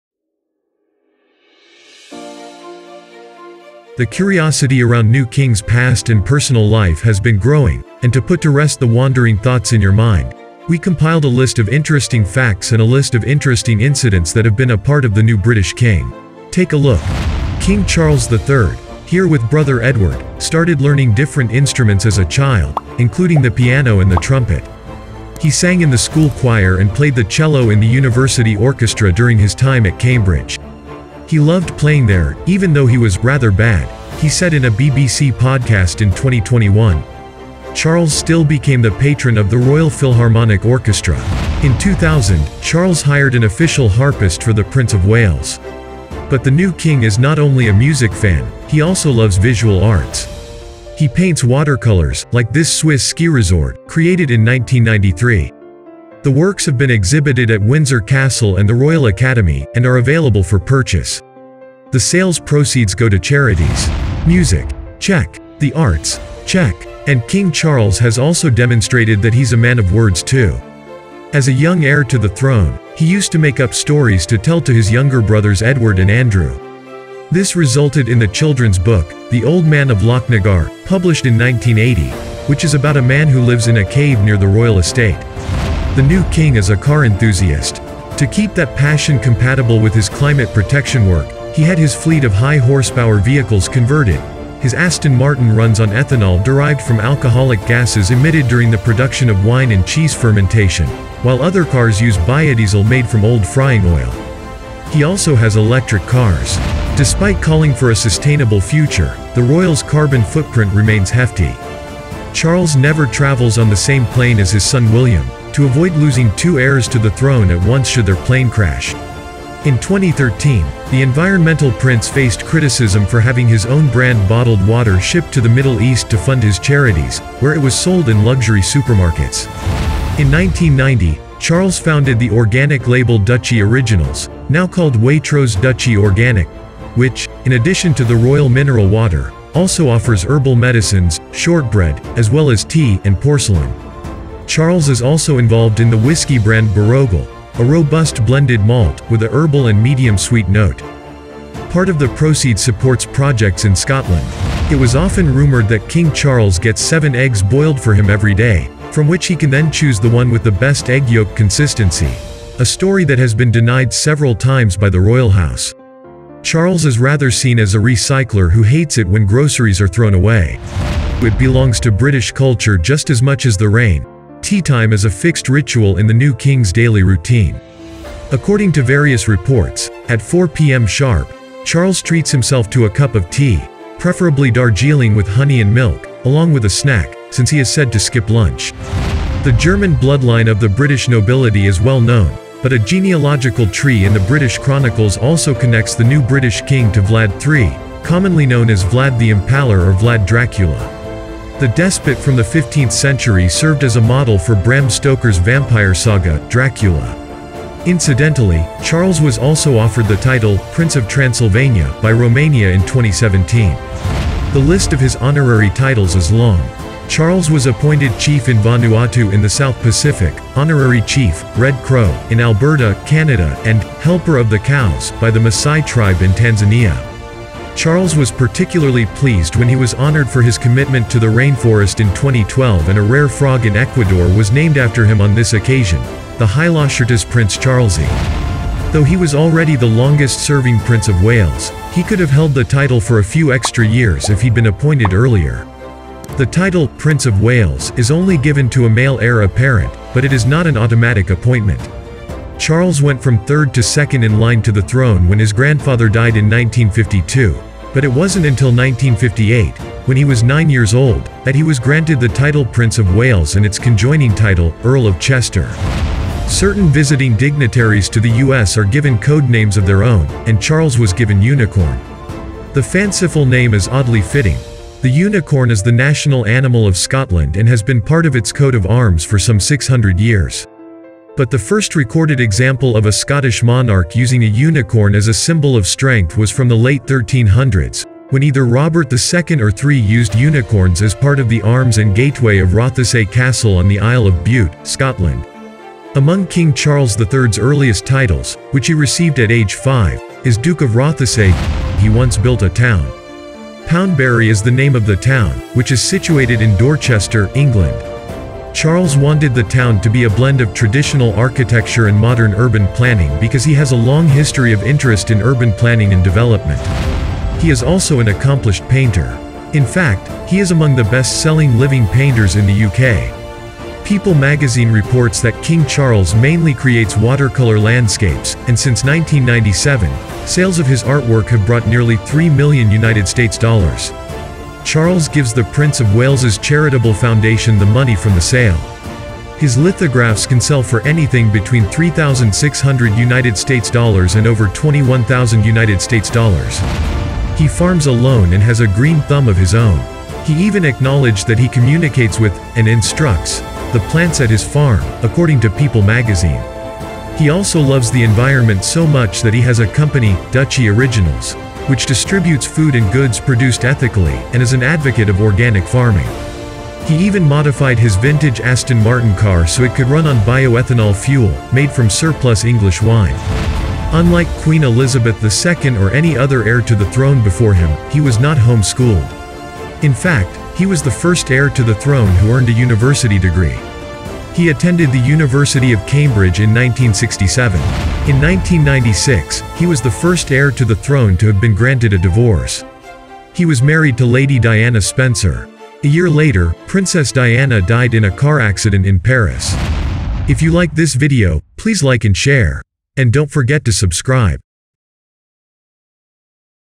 The Truth About King Charles III Will Shock You | What's So Interesting About King Charles III?
Prince Charles is now King Charles III. Here's what to know about UK's new Monarch. After more than 70 years of Queen Elizabeth II's steady reign over the United Kingdom, Prince Charles ascended the Throne upon her death and is now King Charles III.
TIME STAMPS
0:00 - Intro
0:17 - A musical talent
0:41 - A friend of the arts
1:04 - The storyteller
1:26 - A car running on used cooking oil
1:46 - A huge carbon footprint
2:10 - Shortbread and whisky
2:35 - Royal rumors
2:51 - Stylish tea time
3:09 - A descendant of Dracula
3:34 - A frog's name in his honor
3:58 - The Title of Prince of Wales
4:26 - Secret Service nickname
5:02 - He once built a town!
5:17 - Best-Selling Living Painter
5:47 - King Charles loves farming and gardening!
6:14 - Not home-schooled
6:28 - Princess Diana
Background Music Scrore by :